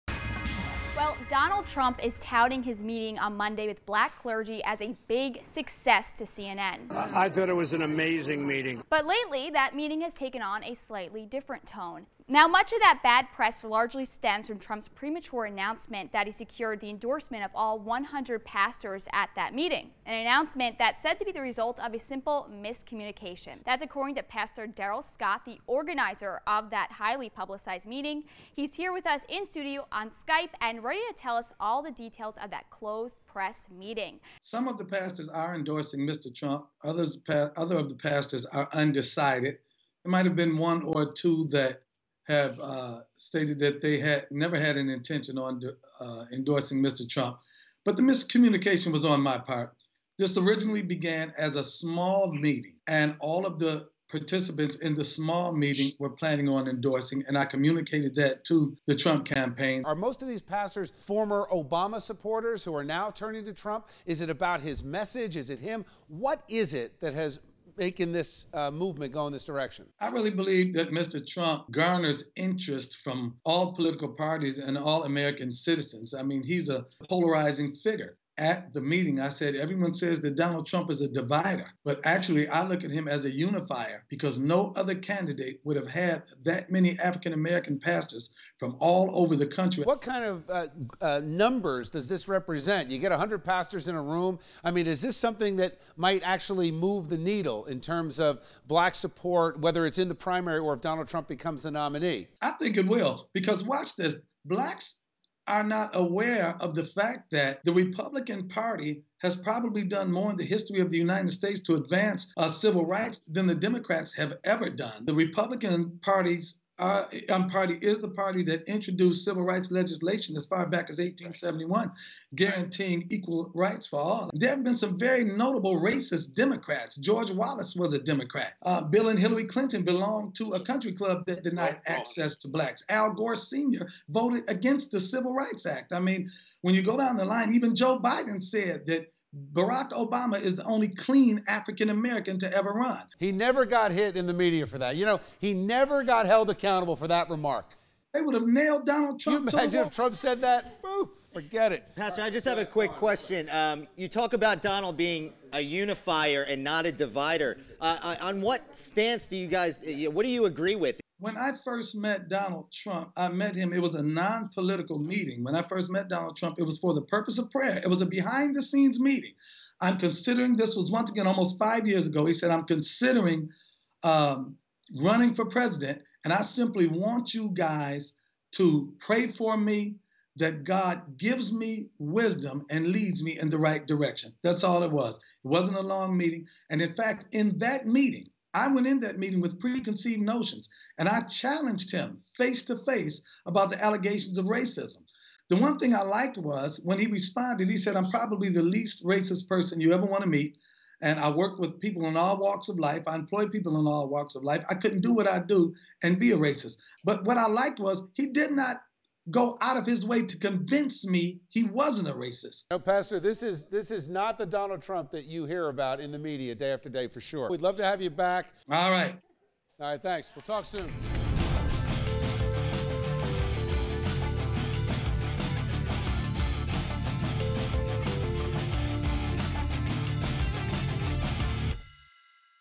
The Pastor Votes Trump
They're called sellouts and prostitutes, ridiculed for meeting with Donald Trump.

But, that's not changing the minds of black clergy members choosing to endorse Trump for president.

"We've had policy. We've had the Great Society, we had Civil Rights, we had Affirmative Action. We had all those things and we're worse. We have a breakdown of morality and Trump is the man to heal that," said Pastor James David Manning of Atlah Church in Harlem.

The bad press largely stems from Trump's announcement that he had secured the endorsement of all 100 pastors scheduled to attend the meeting. Turns out, not all of the pastors planning to attend would endorse the GOP candidate.

"The miscommunication was on my part," said Pastor Darrell Scott, the organizer of the meeting.  "It originally began as a small meeting. Once word got out this meeting was going to happen we got phone calls and friends and friends who requested to attend. When I turned in the list of attendees, I didn't let the campaign know everyone who was coming did not intend to endorse."

Scott said Monday's meeting was a great success.